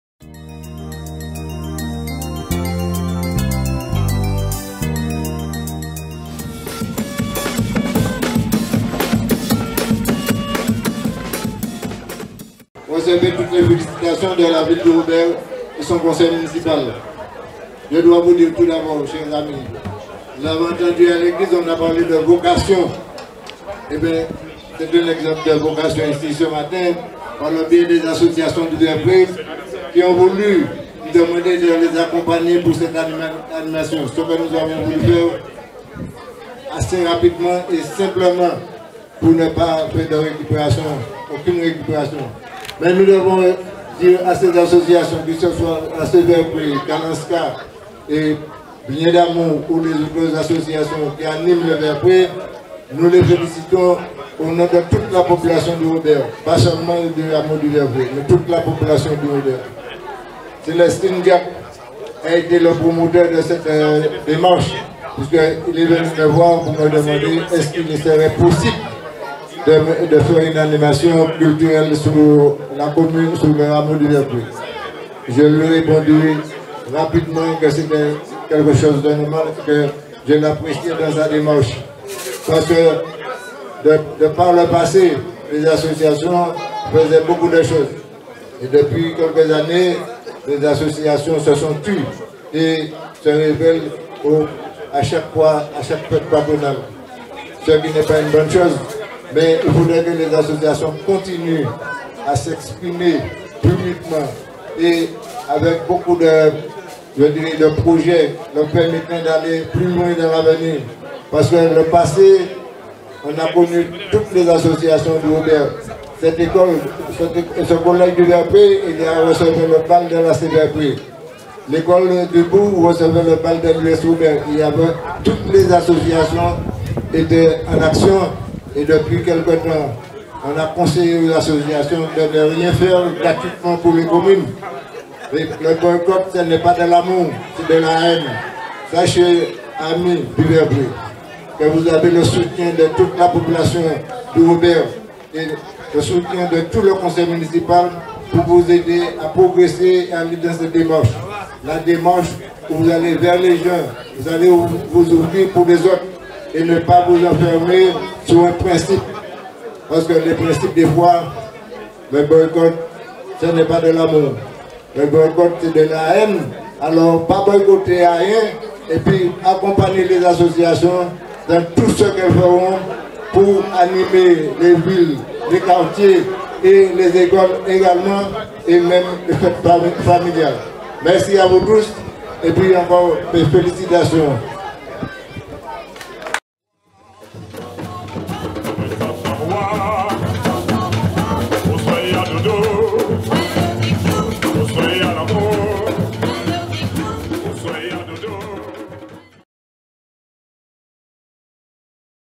Dimanche 12 mai 2019 - Weekend culturel des associations du Vert-Pré
Ce dimanche 12 mai 2019, à l'occasion du weekend culturel des associations du Vert-Pré, à l'issue de la messe et du dépôt de gerbe, le Maire de la ville du Robert Alfred Monthieux accompagné des membres du conseil municipal a prononcé un discours. Retrouvez la vidéo de ce moment.